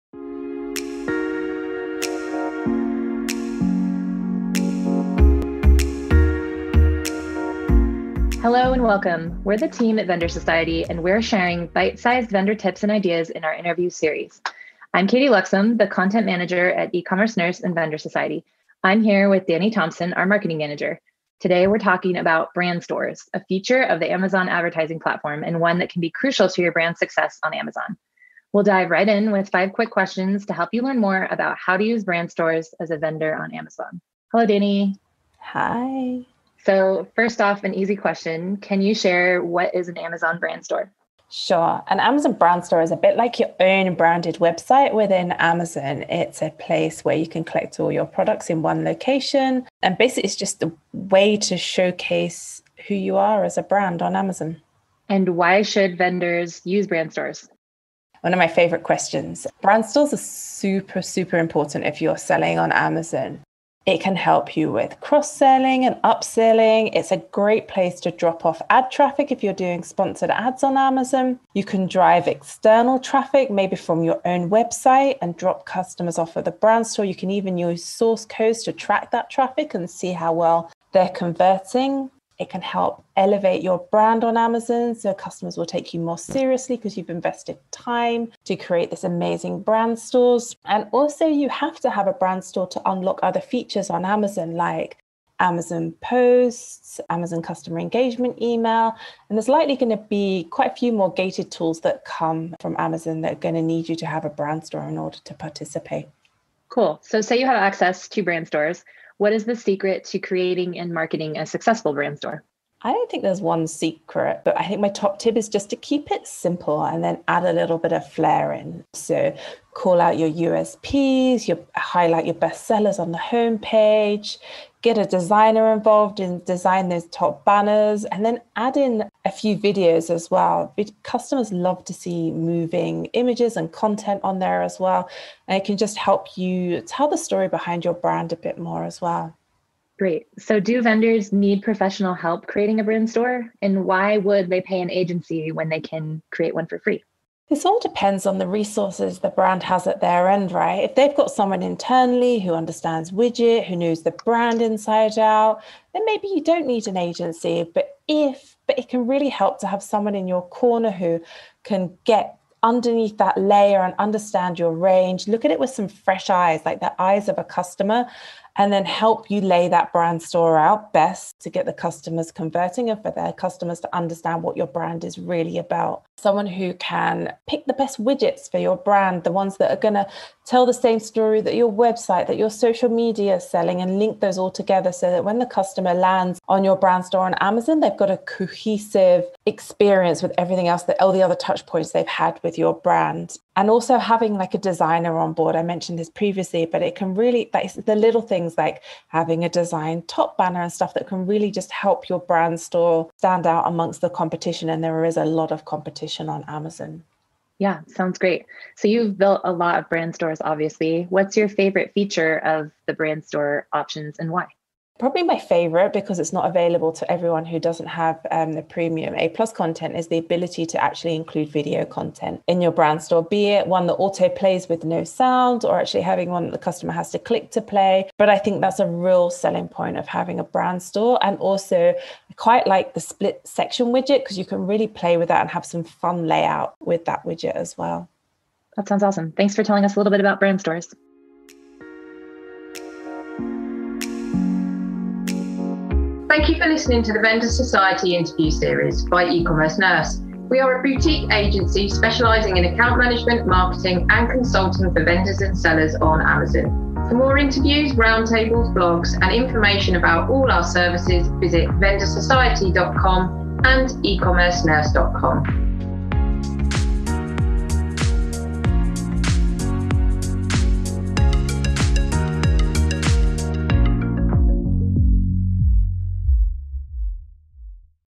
What are Amazon Brand Stores & how should vendors use them?
In this video of the Vendor Society Interview Series, we're discussing Amazon Brand Stores. As a component of the Amazon Advertising platform and a feature of the Brand Registry, there's a lot that vendors (and sellers!) can do with their virtual storefront on Amazon. Use your space and resources wisely! Learn some quick and easy tips with five questions about Brand Store basics, featuring our marketing and content experts.

Listen to bite-size vendor tips and ideas in the Vendor Society Interview Series from the team at eCommerce Nurse.

THIS VIDEO FEATURES:

Dani Thompson - Senior Marketing Manager for Vendor Society and eCommerce Nurse
Katy Luxem - Senior Content Manager for Vendor Society and eCommerce Nurse